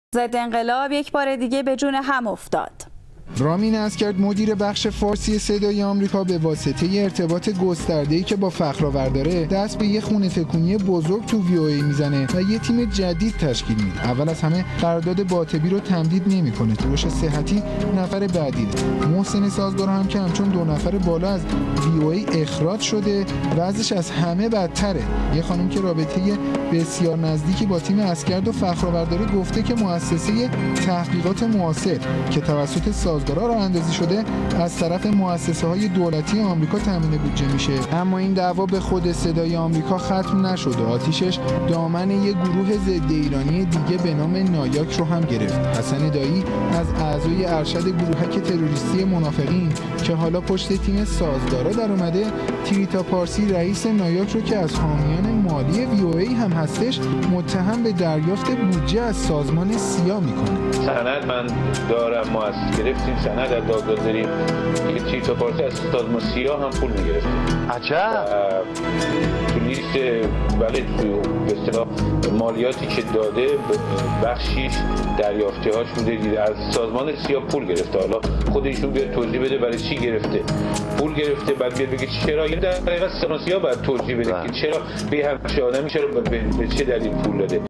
Iranian television throw dirt on VOA Persian TV and repeat story of CIA payment to NIAC Trita Parsi
In its news bulletin of Saturday 3 December , the Iranian TV promote stories against some  Iranian opposition figures in the United States , and air a part of an interview of Hasan Daei in which he blame the founder of National Iranian American Council NIAC  Trita Parsi of taking money from the Central Intelligence Agency CIA .